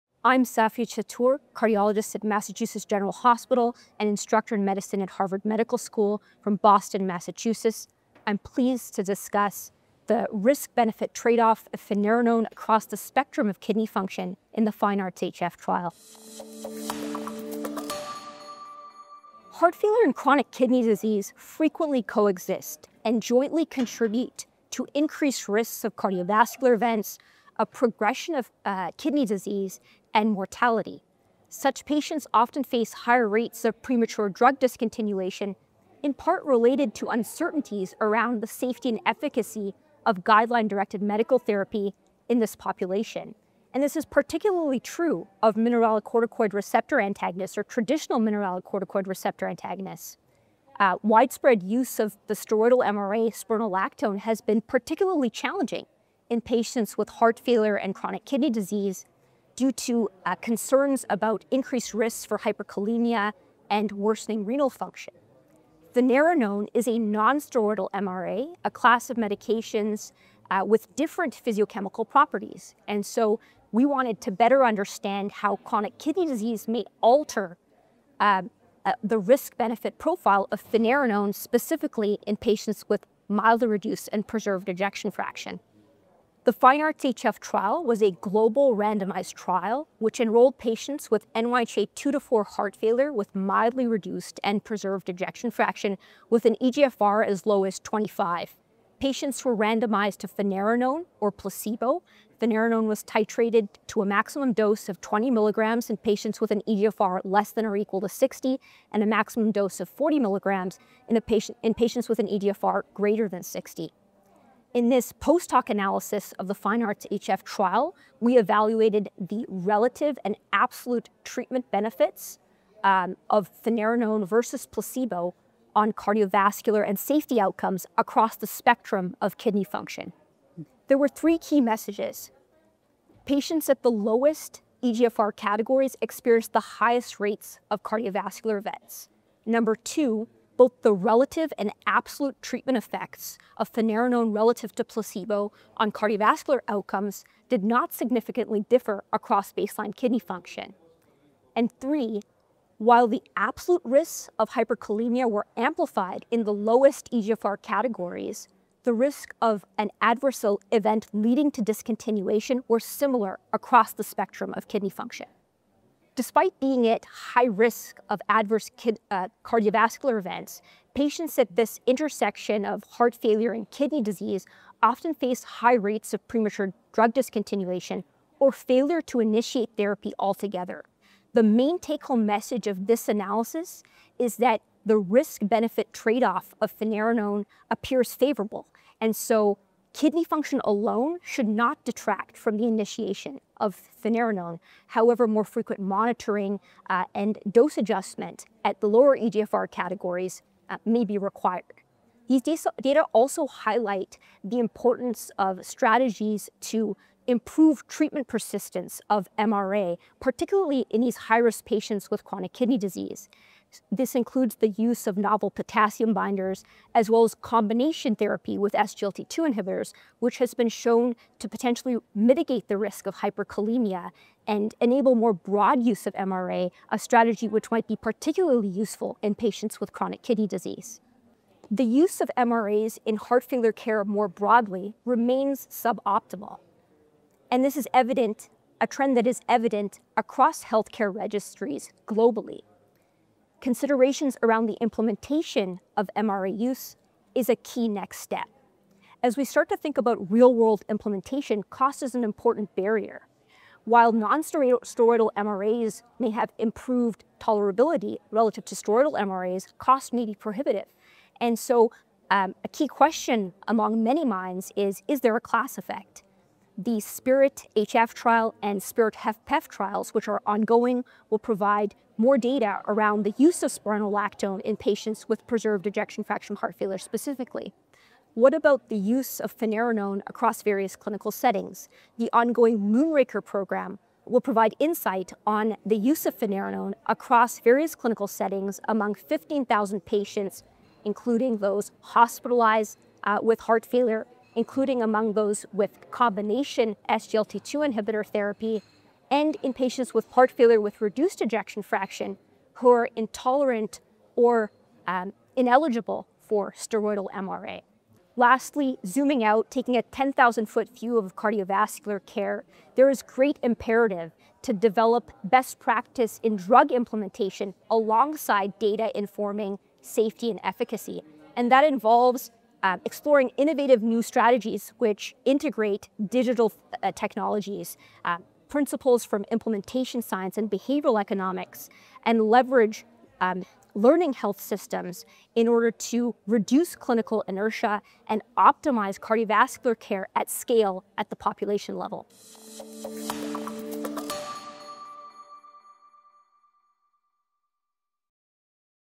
ESC HF 25: FINEARTS-HF: Finerenone Risk-Benefit Across Kidney Function
ESC HFA 25 - In a FINEARTS HF post-hoc analysis, the relative and absolute effects of finerenone relative to placebo did not significantly differ across kidney function.

We are joined by Dr Safia Chatur (Mass General Cardiology, US) to discuss key findings from the FINEARTS-HF trial. This Phase 3 study evaluated the effect of finerenone versus placebo on cardiovascular death and total heart failure events in 6,016 patients with heart failure with mildly reduced and preserved ejection fraction (HFmrEF/HFpEF, EF ≥40%). This post-hoc analysis evaluated the relative and absolute treatment benefits of finerenone vs placebo on cardiovascular and safety outcomes across the kidney spectrum.

Findings showed that patients at the lowest eGFR categories experienced the highest cardiovascular events, and the relative and absolute effects of finerenone relative to placebo did not significantly differ across kidney function. Though absolute risk of hyperkalemia was amplified in those in the lowest eGFR categories, the risk of an adverse event leading to discontinuation were similar across the spectrum of kidney function.

Interview Questions:

1. What was the rationale for investigating finerenone specifically across the spectrum of kidney function in patients with HFmrEF/HFpEF?
2. Could you describe the study design and patient population of FINEARTS-HF, particularly focusing on the kidney function subgroups analyzed?
3. What were the key findings regarding the efficacy and safety of finerenone across different levels of kidney function?
4. What are the clinical implications of these results for managing patients with HFmrEF/HFpEF who have varying degrees of kidney function?
5. What further research questions emerge from these findings, and what next steps would you recommend in this therapeutic area?

Recorded at the European Society of Cardiology Heart Failure Association Congress 2025.

Editors: Yazmin Sadik, Jordan Rance
Videographer: Tom Green, Oliver Miles

Support: This is an independent interview produced by Radcliffe CVRM.